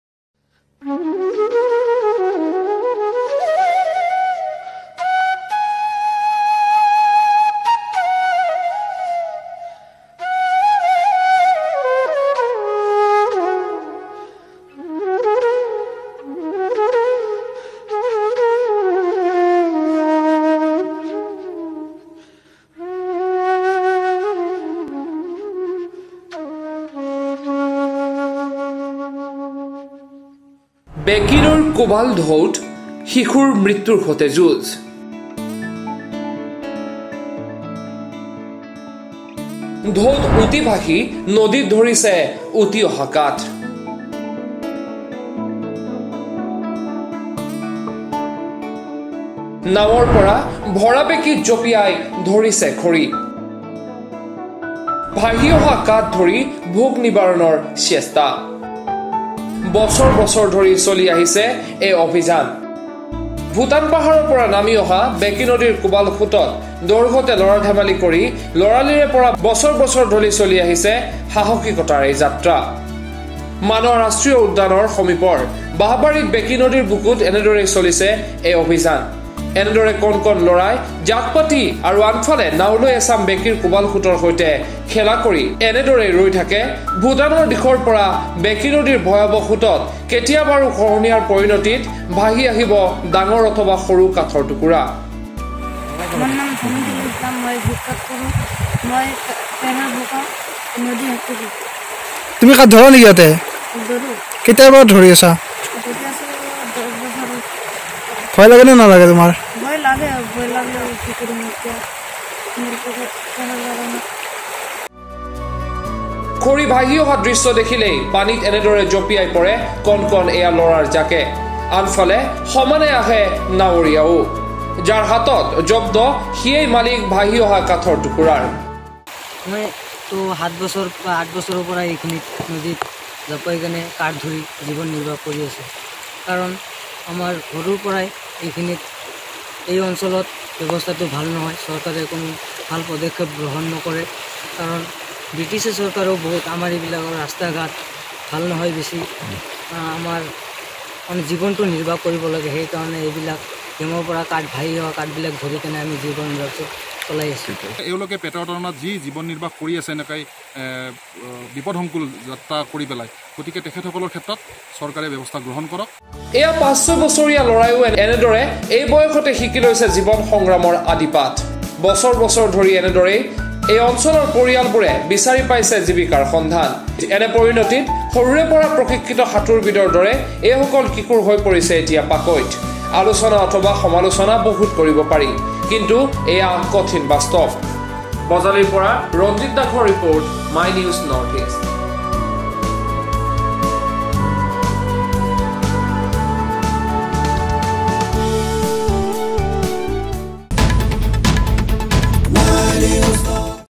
বেকী নৈৰ কোবাল ঢৌত কিহৰ সন্ধানত এজাক শিশু? সবিশেষ চাওক এই প্ৰতিবেদনত...
My News NE:  বেকী নৈৰ কোবাল ঢৌত কিহৰ সন্ধানত এজাক শিশু?
সবিশেষ চাওক এই প্ৰতিবেদনত...